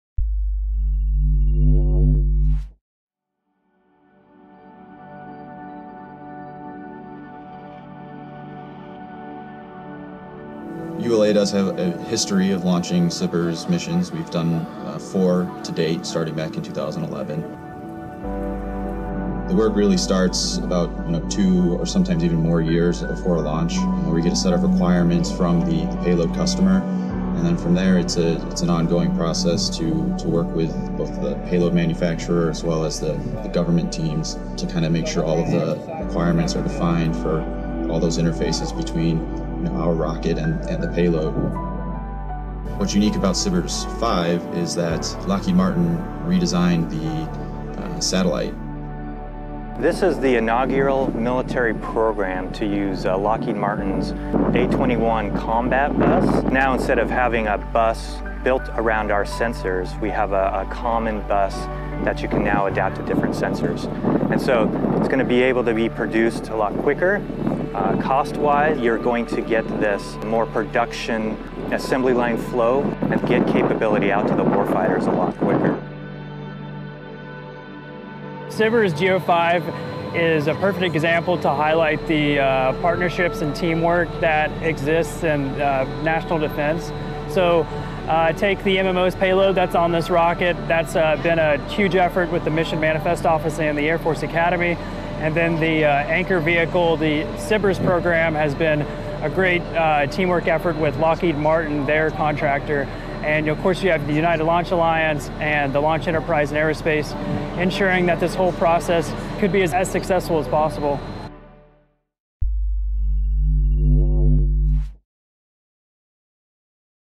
SBIRS GEO-5 highlights partnership
EL SEGUNDO, CA, UNITED STATES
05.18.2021

The SBIRS GEO-5 launch at Cape Canaveral, Fla., May 18, 2021, highlighted the importance of partnerships between the U.S. Space Force, Lockheed Martin, United Launch Alliance and more.

Film Credits: Video by Staff Sgt. Luke Kitterman
Space and Missile Systems Center Public Affairs

Lockheed Martin
USSF
ULA